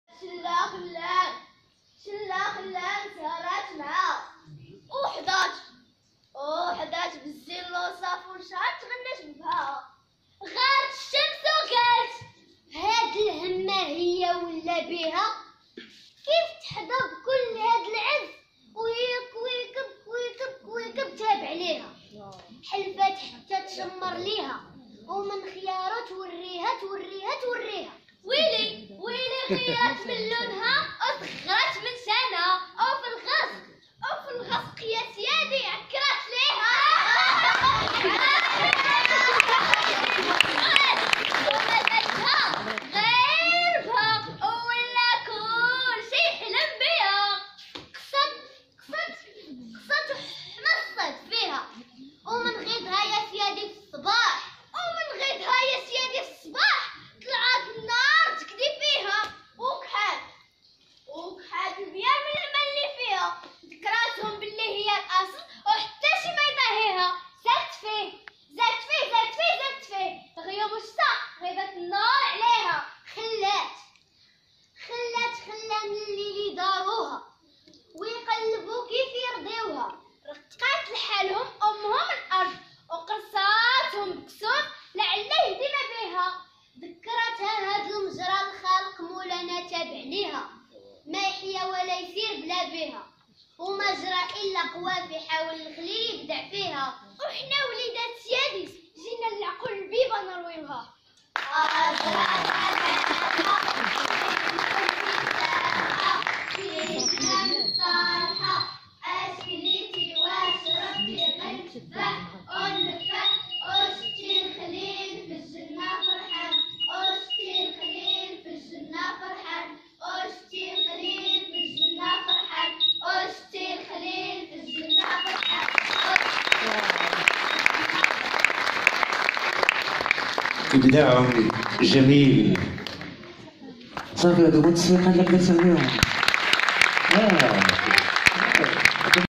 براعم أبدعت في مشهد القمر والشمس، اخراج الفنانة عائشة رشدي / إهداء لروح الخليل.
مسرحية من انجاز الأستاذة عائشة رشدي والمجموعة المسرحية.